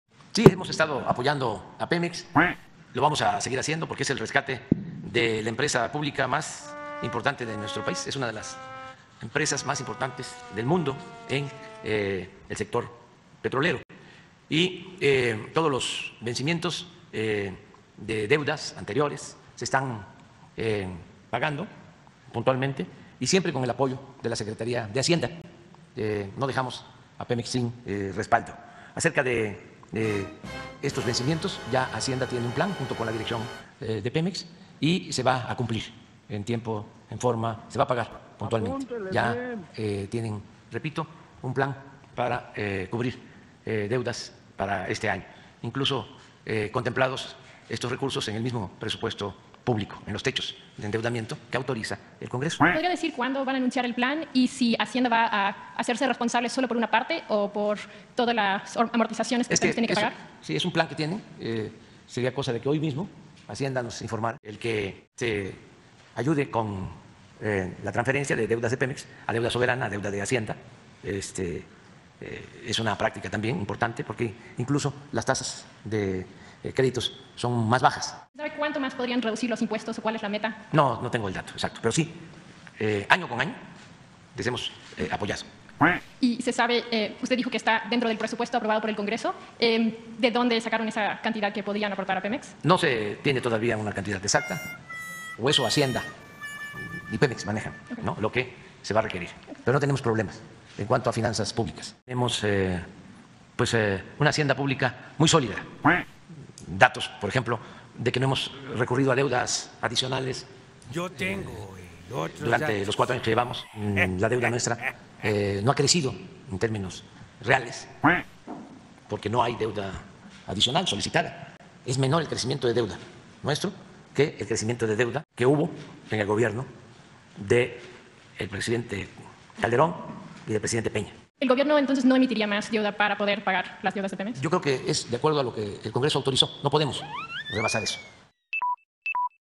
Seguiremos con el rescate de Pemex, Hacienda ya tiene un plan para cubrir los vencimientos de deuda🦆
Seguiremos con el rescate de Pemex, empresa pública más importante del país y del mundo del sector petrolero, Hacienda ya tiene un plan para cubrir los vencimientos de deuda 🦆